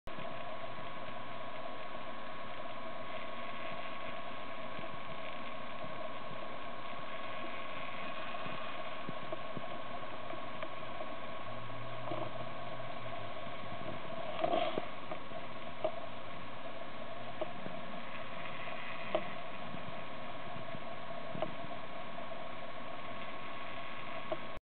Dinosaur Bone Cabochon Henry Mts. Southern Utah Large
Dinosaur Bone Cabochon, Material collected Henry Mountains, Southern Utah. Great unusual colors. Measures 66mm x 29mm.
For sale on eBay under seller ID ( imaginative_gems ).